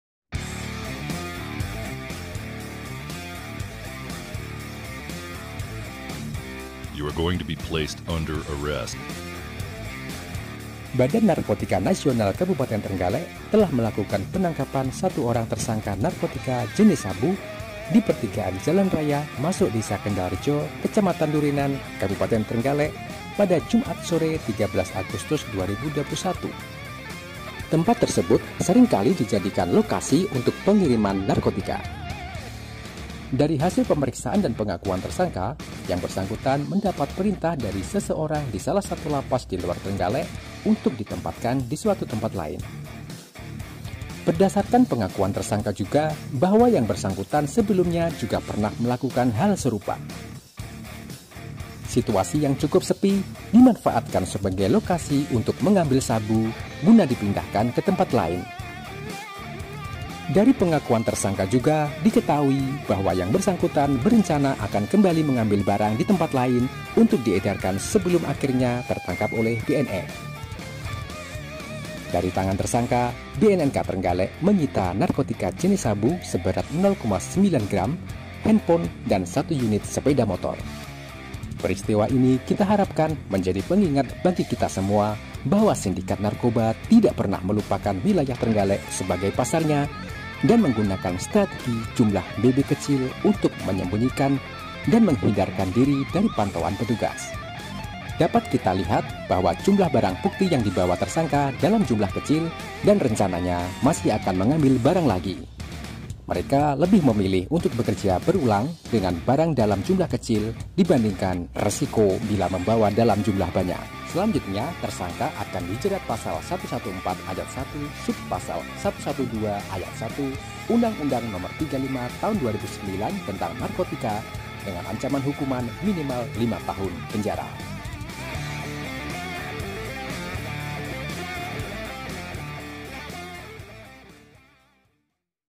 Edarkan Sabu di Trenggalek, Kurir Narkoba ini di Tangkap BNNK Trenggalek
Badan Narkotika Nasional Kabupaten Trenggalek telah melakukan penangkapan 1 orang tersangka narkotika jenis shabu di pertigaan jalan raya masuk desa kendalrejo kecamatan Durenan kabupaten Trenggalek pada Jumat sore 13 Agustus 2021.  Tempat tersebut seringkali dijadikan sebagai lokasi untuk pengiriman narkotika.

Dari hasil pemeriksaan dan pengakuan tersangka, yang bersangkutan mendapat perintah dari seseorang disalah satu lapas di luar trenggalek, untuk ditempatkan di suatu tempat lain.

Berdasarkan pengakuan tersangka juga, bahwa yang bersangkutan sebelumnya juga pernah melalukan hal serupa.

Situasi yang cukup sepi dimanfaatkan sebagai lokasi untuk mengambil shabu guna dipindahkan ke tempat lain. Dari pengakuan tersangka juga diketahui bahwa yang bersangkutan berencana akan kembali mengambil barang ditempat lain untuk diedarkan sebelum akhirnya tertangkap oleh BNN

Peristiwa ini kita harapkan menjadi pengingat bagi kita semua bahwa sindikat narkoba tidak pernah melupakan wilayah trenggalek sebagai pasarnya, dan menggunakan strategi jumlah BB kecil untuk menyembunyikan dan menghidarkan diri dari pantauan petugas.

Dari tangan tersangka, BNNK Trenggalek menyita narkotika jenis sabu seberat 0,95 gram, handphone, dan satu unit sepeda motor.

Dapat kita lihat bahwa jumlah barang bukti yang dibawa tersangka dalam jumlah kecil dan rencananya masih akan mengambil barang lagi. Mereka lebih memilih untuk bekerja berulang dengan barang dalam jumlah kecil dibandingkan resiko bila membawa dalam jumlah banyak.

Selanjutnya tersangka akan dijerat pasal 114 ayat 1 sub pasal 112 ayat 1 UU No 35 tahun 2009 tentang narkotika dengan ancaman hukuman min 5 tahun penjara.